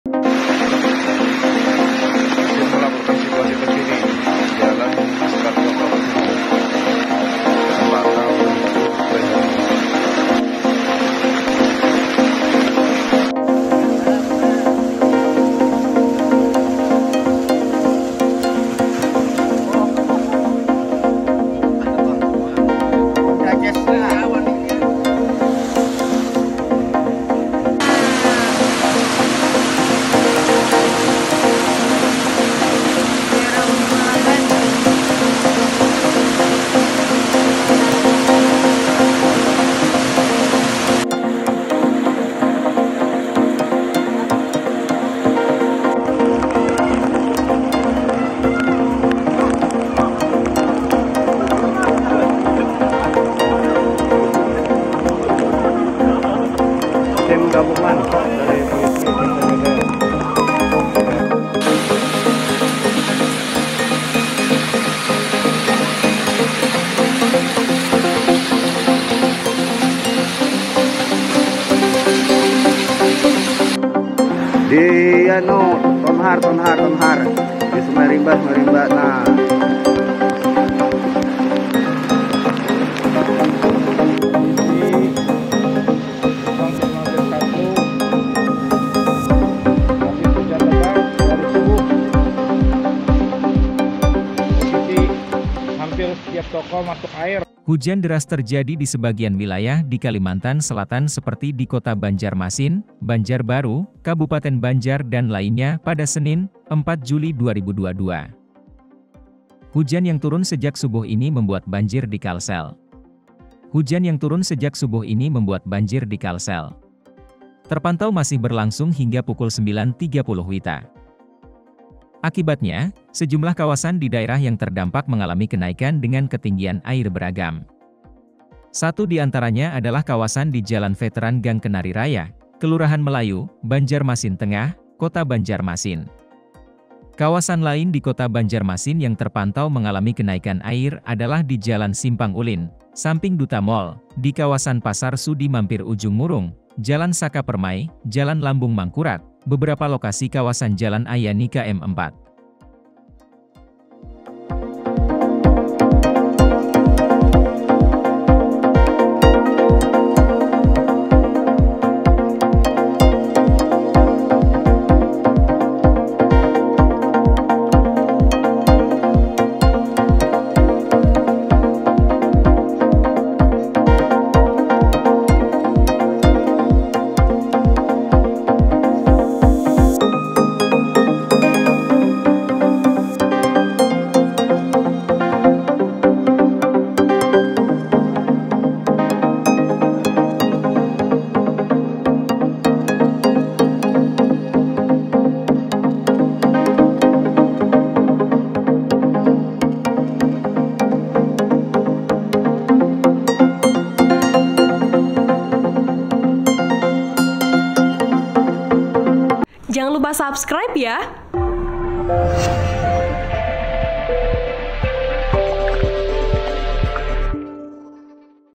Banjir Landa Sejumlah Titik di Kota Banjarmasin Setelah Diguyur Hujan Selama Enam Jam
WARTAKOTALIVE.COM, BANJARMASIN - Guyuran hujan deras yang melanda Kota Banjarmasin dari subuh, Senin (4/7/2022) hingga saat ini belum reda.

Kondisi ini membuat sebagian ruas jalan dan permukiman warga di Banjarmasin terendam banjir.

Begitupula pusat pertokoan yang ada di Pasar Sudimampir tidak luput terendam banjir.

Aair yang menggenangi jalan terus naik hingga masuk kedalam toko yang ada di kawasan Pasar Sudimampir, Banjarmasin.